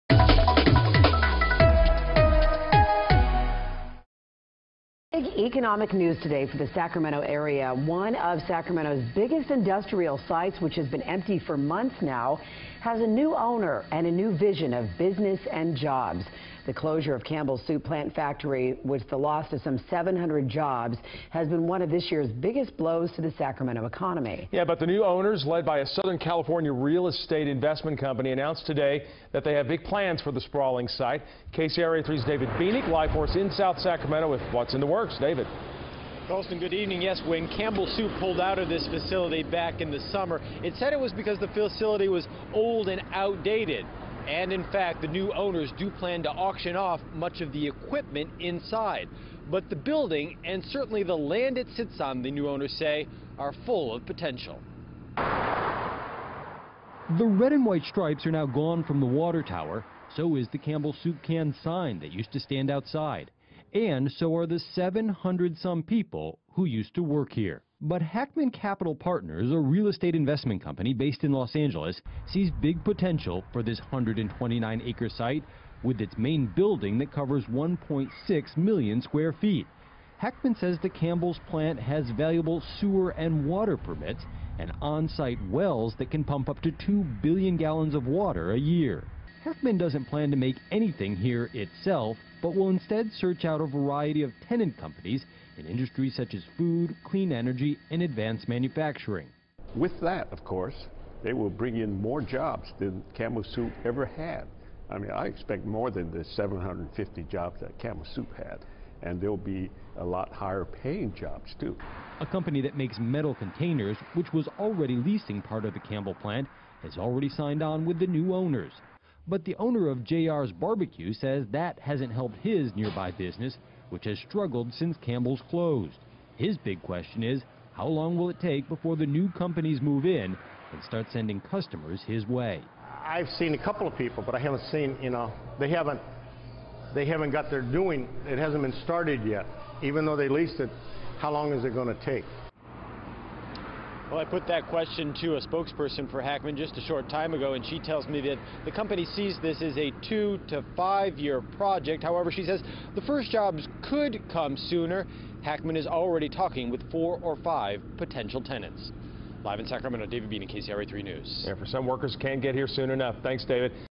New plans in works for old Campbell Soup plant
New owners of the old Campbell Soup plant say they have big plans for the site.